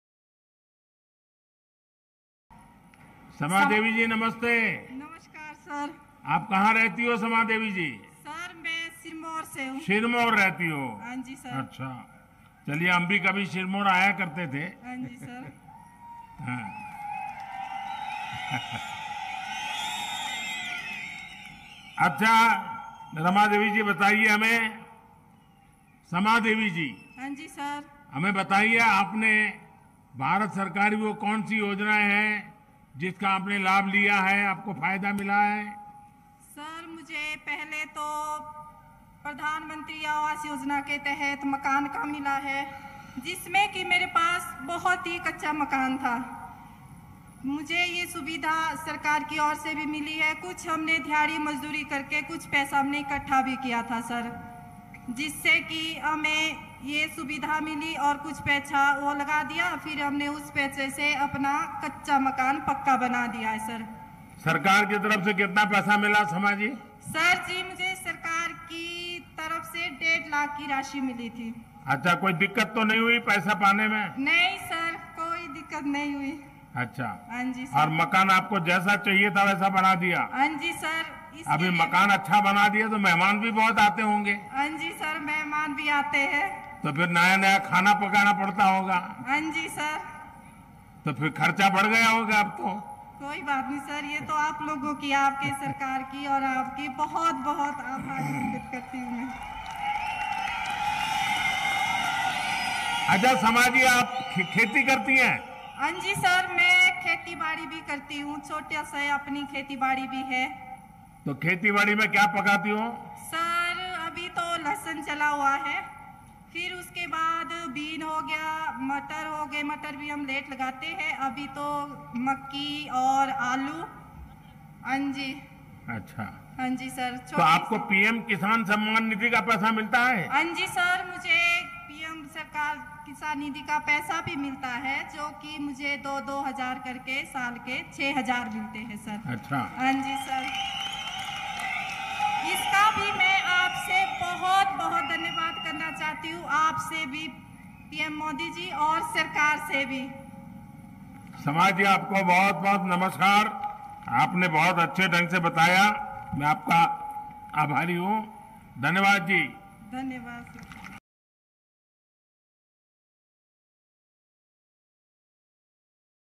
भारत सरकार की योजनाओं पर प्रधानमंत्री जी द्वारा हिमाचल में जन-संवाद | The ForeFront News
हिमाचल प्रदेश में प्रधानमंत्री श्री नरेंद्र मोदी जी ने लोगों से बात की।  भारत सरकार की योजनाओं से उनको किस तरह का लाभ मिला है, ये भी जानने का प्रयास किया। लोग भी उत्साहित नजर आये और उन्होंने ने अपने व्यक्तिगत विचार साँझा किये।